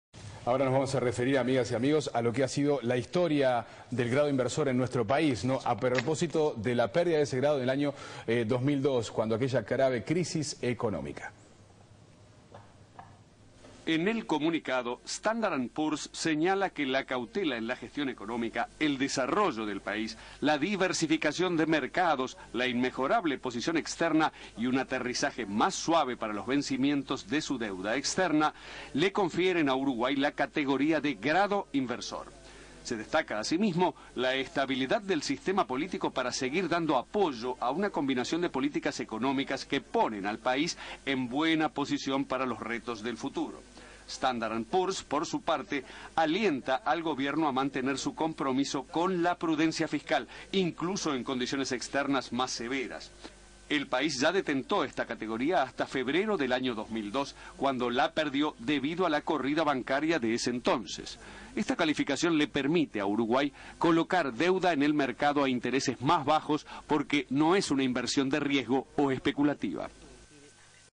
Historia del grado inversor en Uruguay
En su informe, Standard & Poor´s señala que el correcto manejo de la economía de Uruguay permitió una mejora del posicionamiento externo del país, disminuyendo la vulnerabilidad ante situaciones externas adversas. Agrega que confía en que el sistema político y las instituciones uruguayas podrán continuar brindando soporte a la política económica para sobrellevar futuras dificultades.